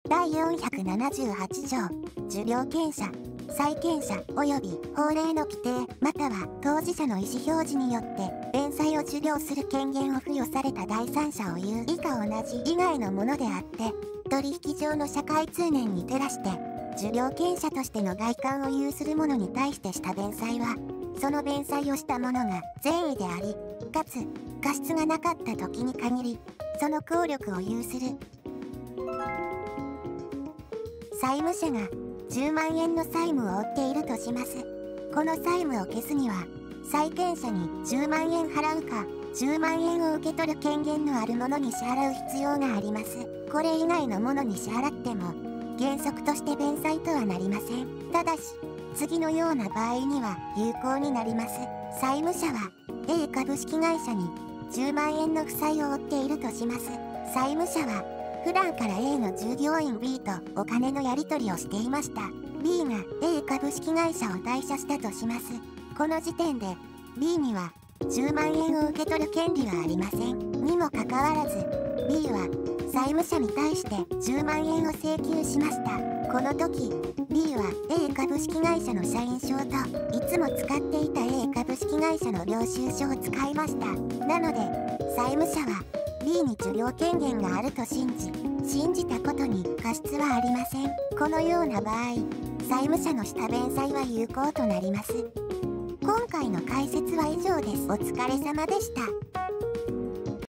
民法を１条から順に解説するよ！ 第４７８条 受領権者としての外観を有する者に対する弁済 【民法改正対応】【ゆっくり・VOICEROID解説】
民法第条の解説です。
この動画は資格習得の役に立てていただけるように作成しています。
完全無料で民法の解説をしています。